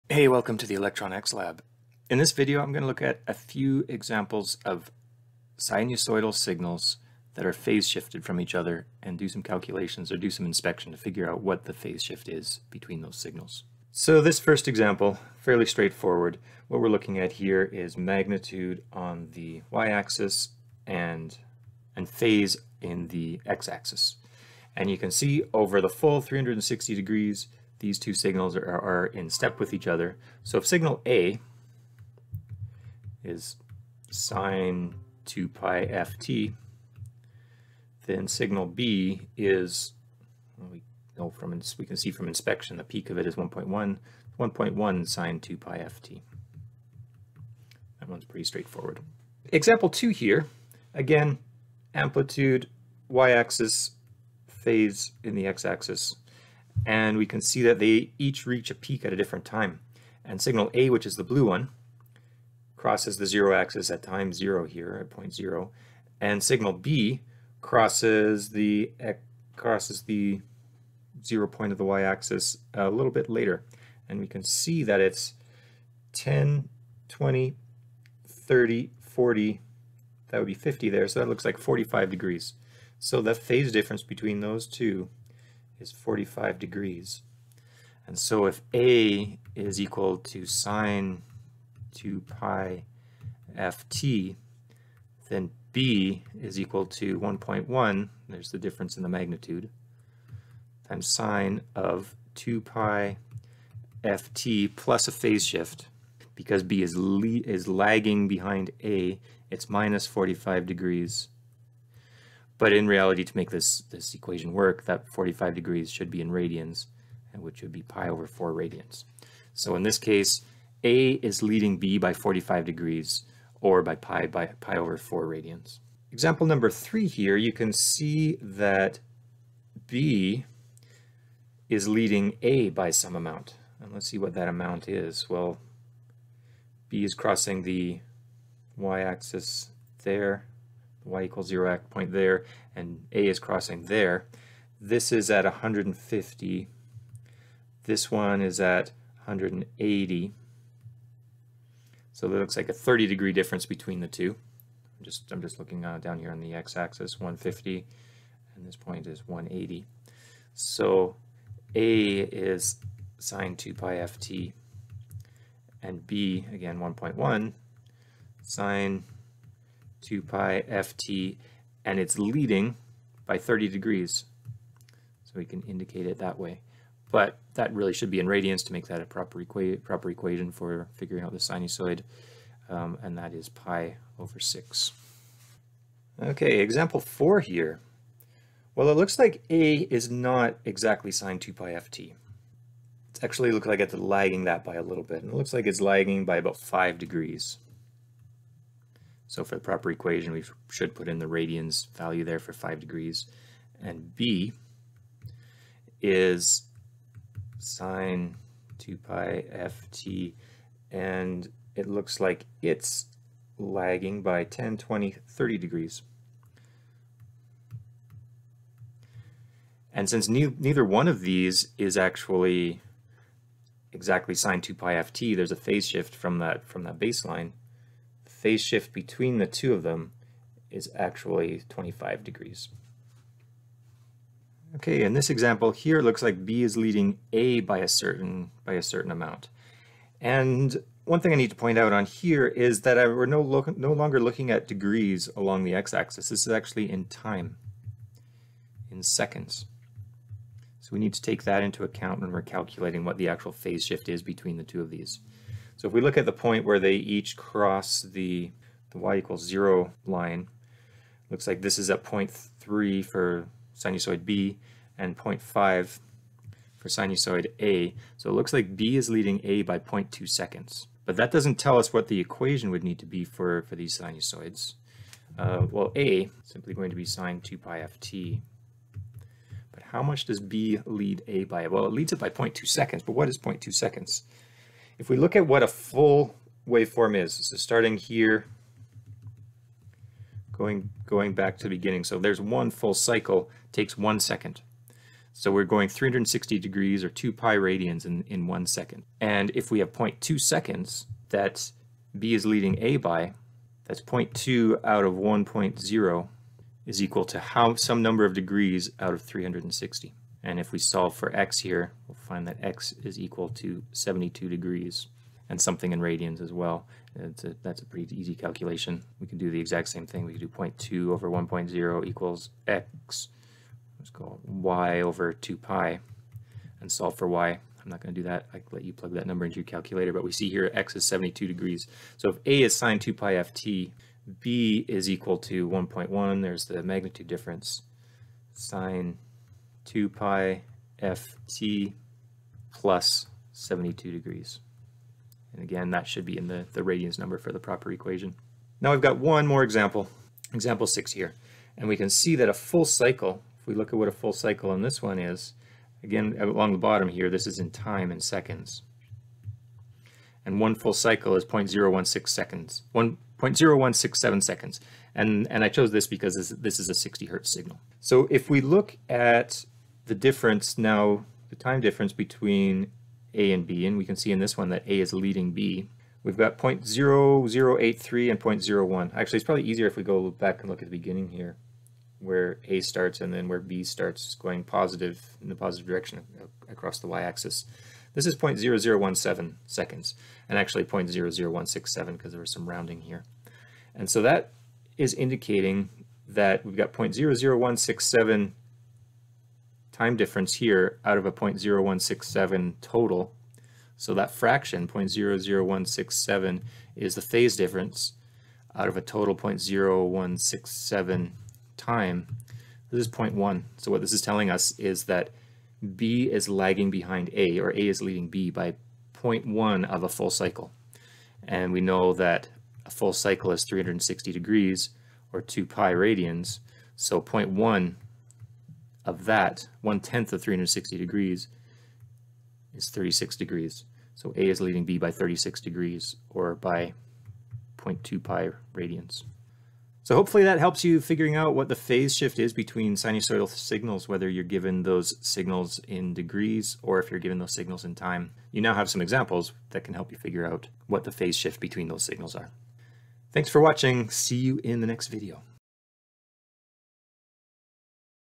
Determining Phase Shift Between Sinusoidal Signals
This video provides a few examples on how to determine phase shift between sinusoidal signals. The examples start with the signals shown as graphs with either time or phase (in degrees) shown on the horizontal axis and determines the phase shift between them/

00:15 - Example 1. No phase shift
00:50 - Example 2. A leads B and horizontal axis shown in degrees
02:05 - Example 3. A lags B and horizontal axis shown in degrees
03:05 - Example 4. Horizontal axis shown in degrees
03:54 - Example 5. Horizontal axis shown in time. Frequency is 1 Hertz
05:50 - Example 6. Horizontal axis shown in time. Frequency is 60 Hertz